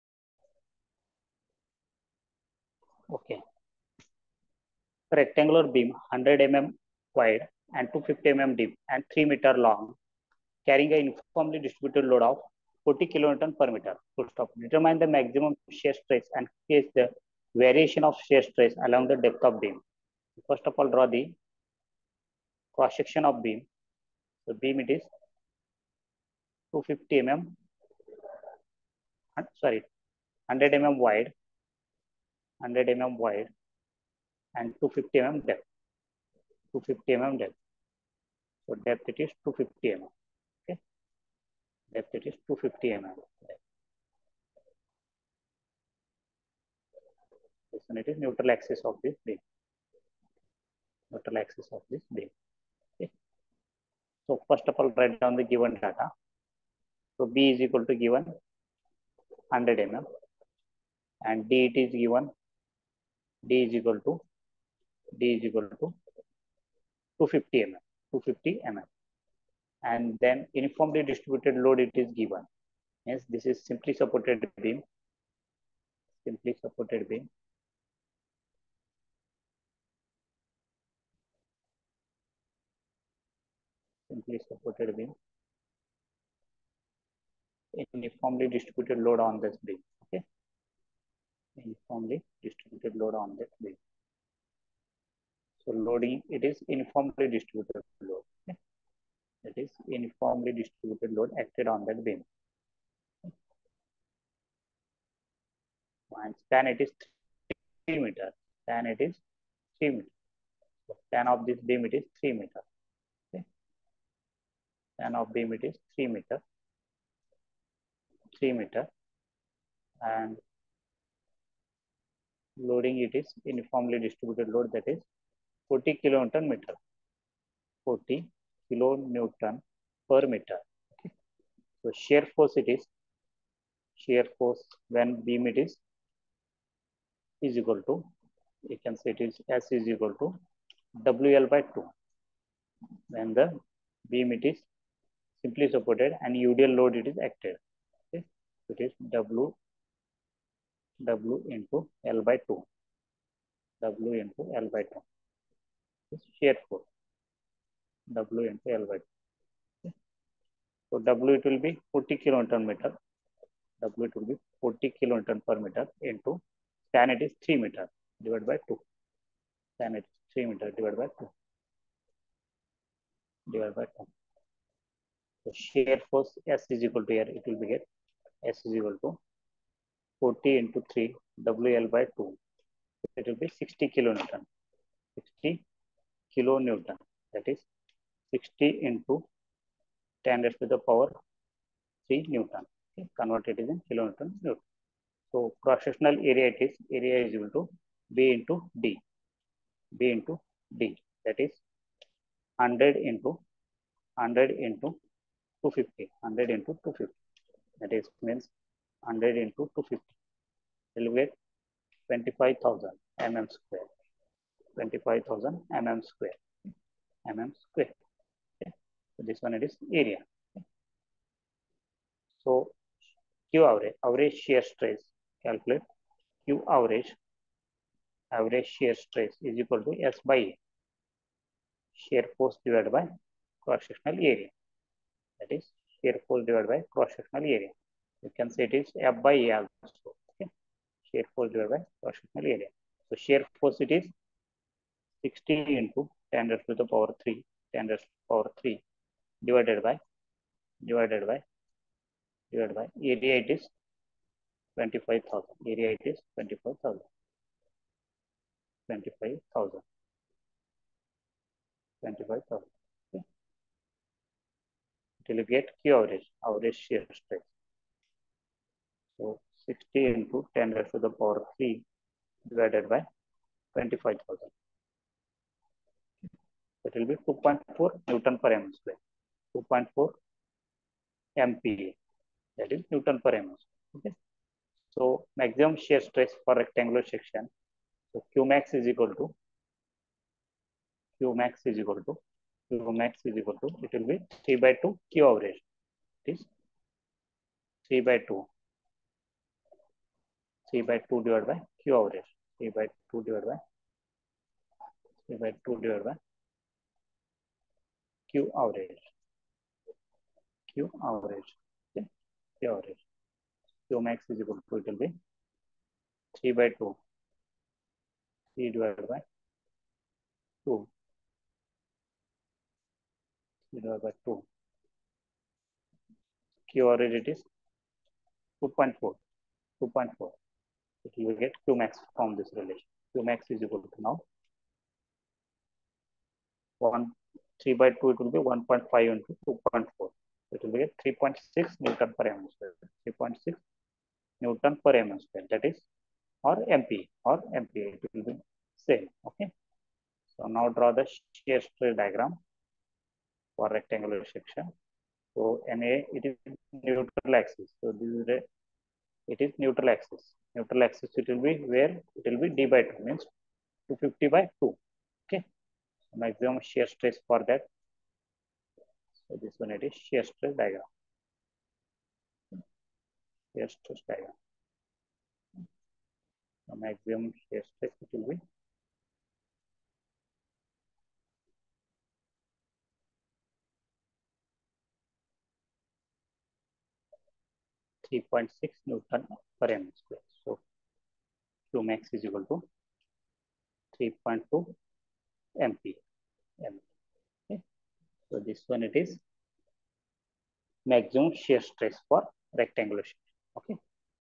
Maximum shear stress for rectangular section/strength of materials
A wooden beam 100mm wide and 250mm deep and 3m long is carrying uniformly distributed load of 40 kN/m. Determine the maximum shear stress and sketch the variation of shear stress along the depth of beam